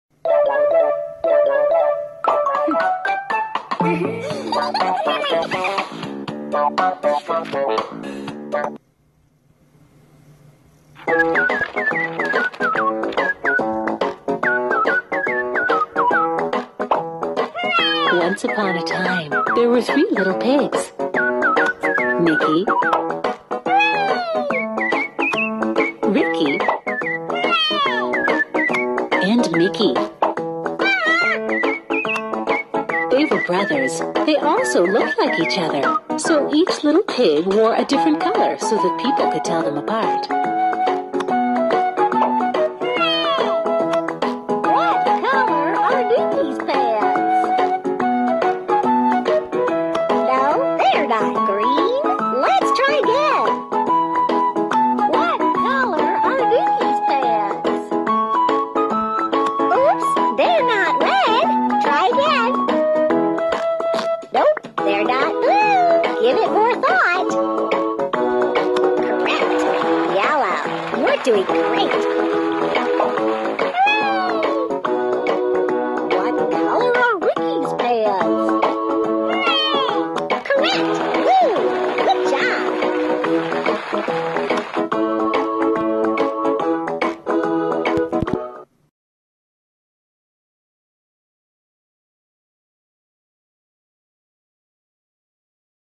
Three Little Pigs - App!
Our app "Three Little Pigs" is  finished!

The interactive e-learning app "The Adventures of the Three Little Pigs" - is perfectly suitable for young children from the age of one year and upwards. Along with encountering the qualities of a children’s book, children are inspired to independently explore events within the app and to communicate with the app using diverse features.